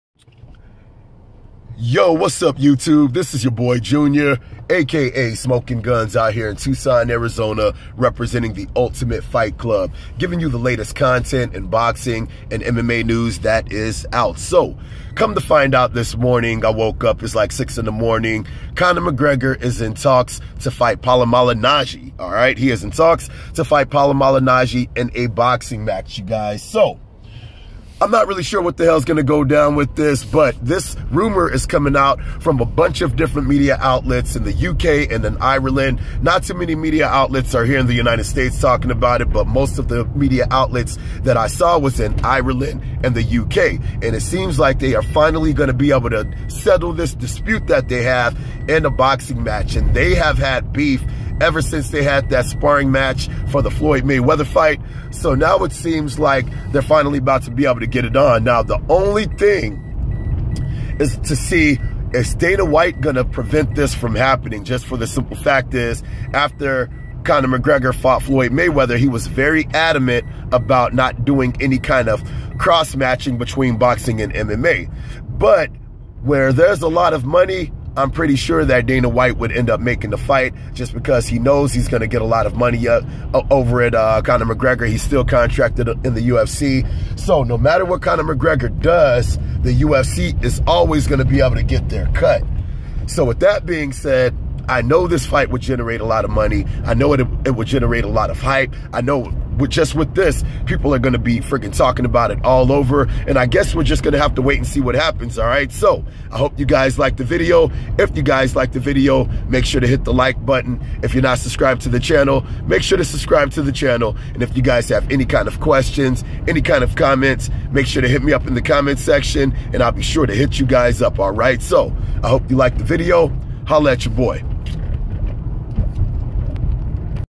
BREAKING NEWS!!! CONOR MCGREGOR IN TALKS TO FIGHT PAULIE MALIGNAGGI IN BOXING MATCH!!!
Media outlets all over are saying  CONOR MCGREGOR is in talks with PAULIE MALIGNAGGI for a potential boxing match!!!

CONOR MCGREGOR vs PAULIE MALIGNAGGI maybe on the horizon!!!